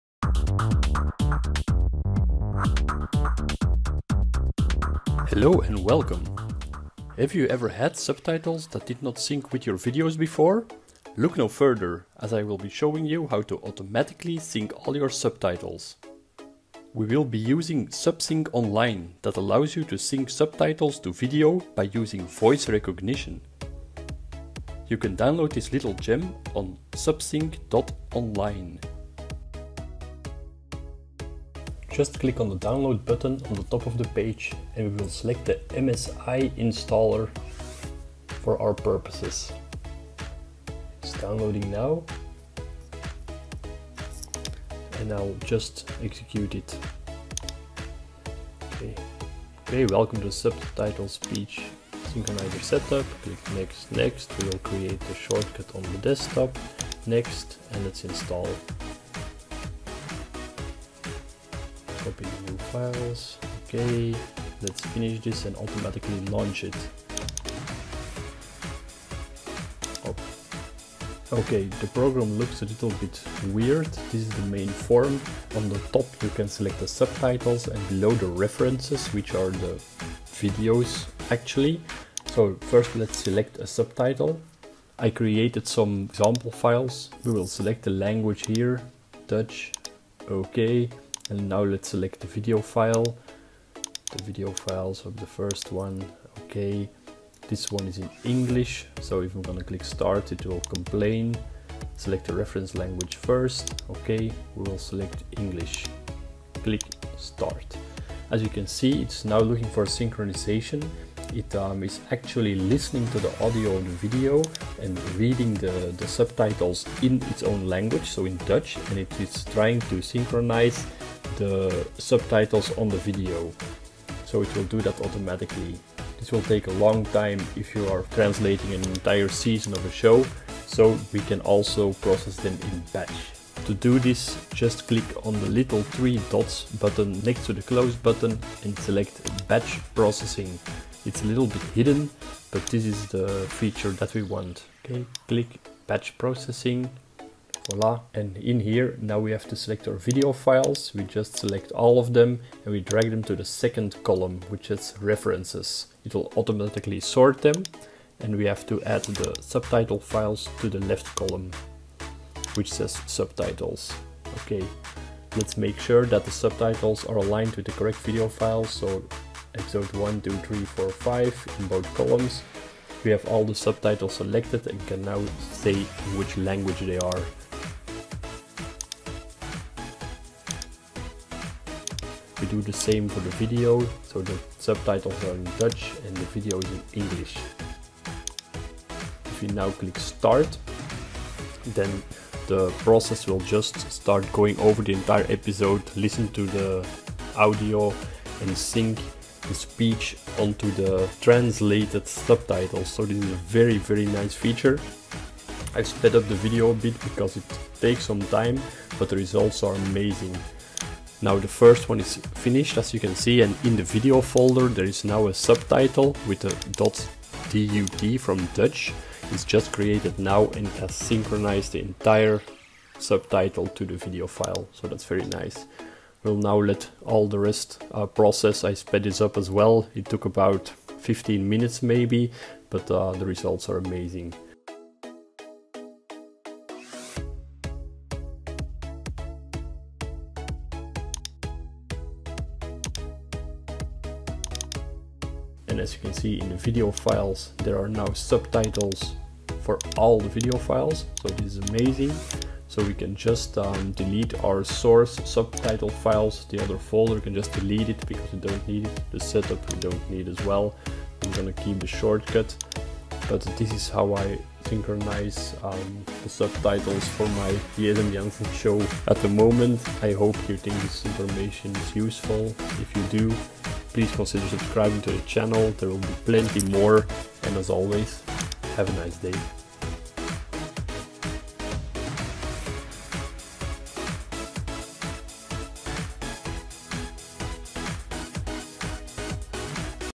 Synchronising subtitles with speech recognition, batch processing and multi language support
I will show you how to synchronize subtitles with video files by using speech recognition and batch processing.

It does not matter what language the subtitles are or the language of the video! You can convert entire seasons in one set!